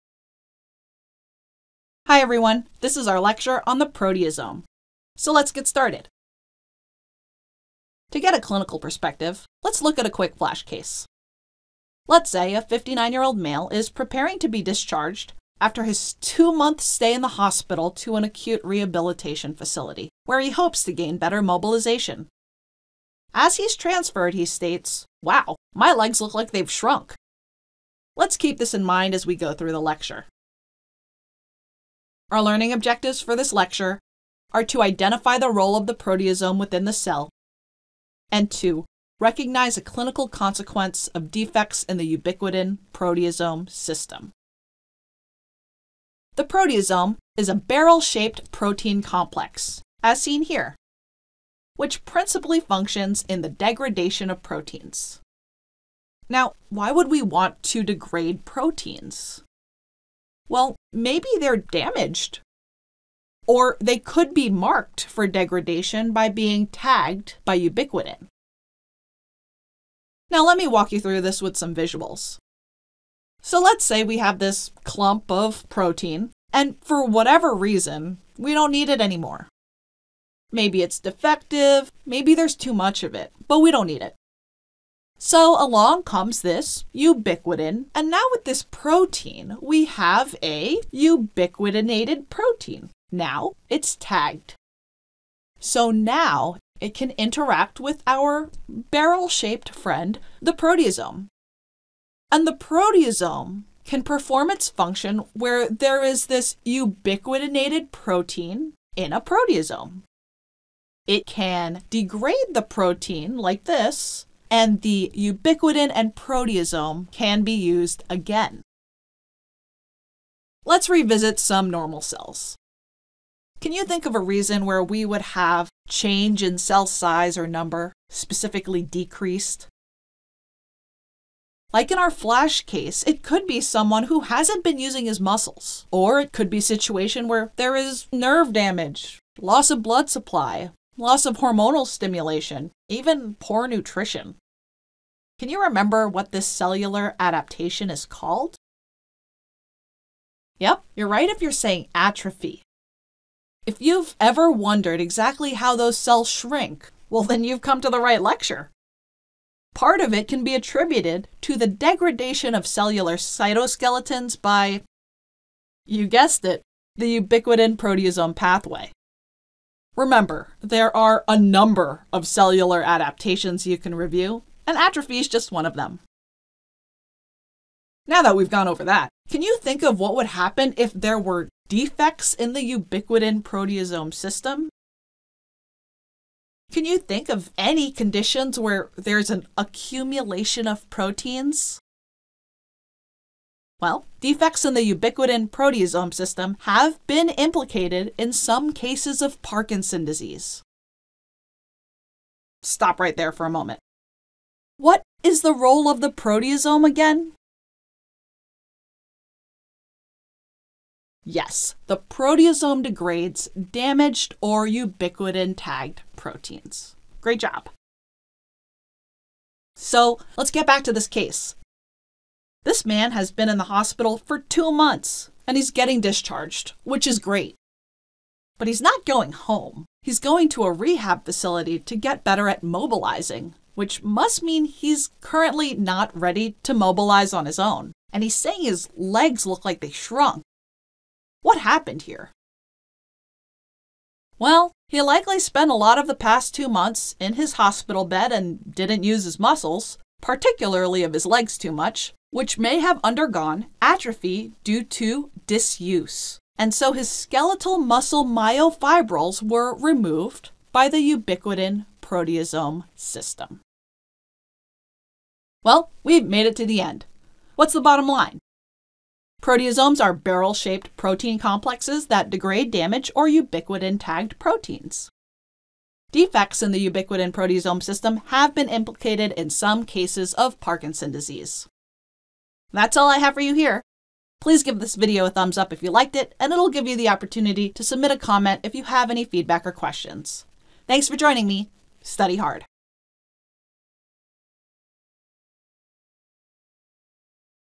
USMLE-Rx Express Video of the Week: Proteasome
Our Express Video of the Week covers Proteasome, from the Cellular section of the Biochemistry chapter in First Aid for the USMLE Step 1.